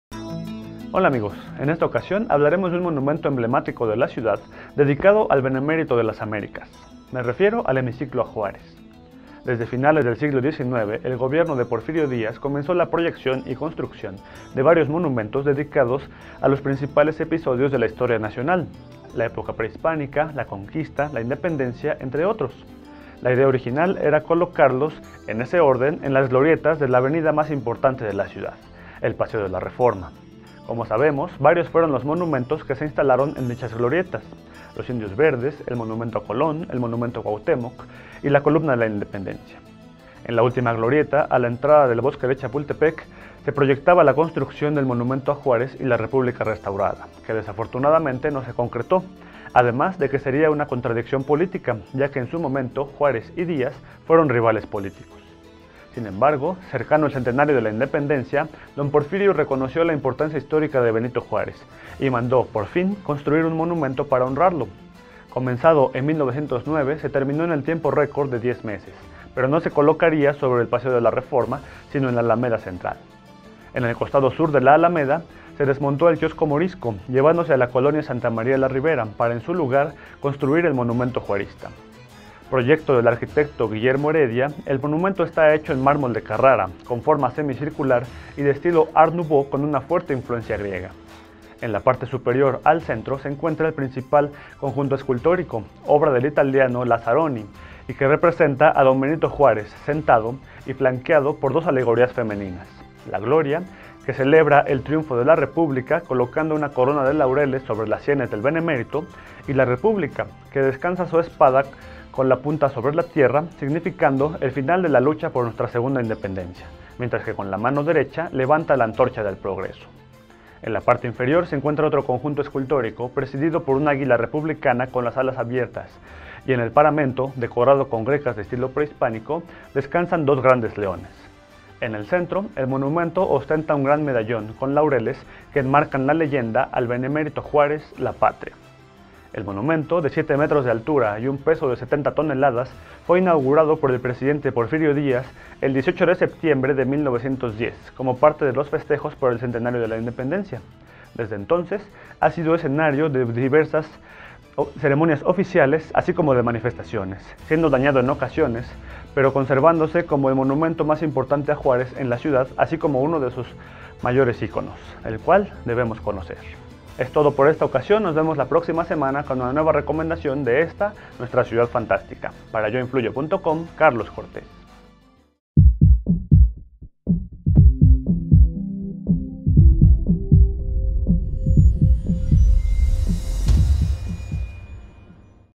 Juárez en la Alameda
Comentario Carlo Cortés / Ideados por el régimen porfirista, desde finales del siglo XIX, se construyeron varios monumentos conmemorativos de las distintas épocas históricas de México; uno de los últimos, fue el dedicado a Benito Juárez y la República Restaurada. Hecho en tiempo récord, se inauguró en la Alameda Central, desplazando al kiosco morisco.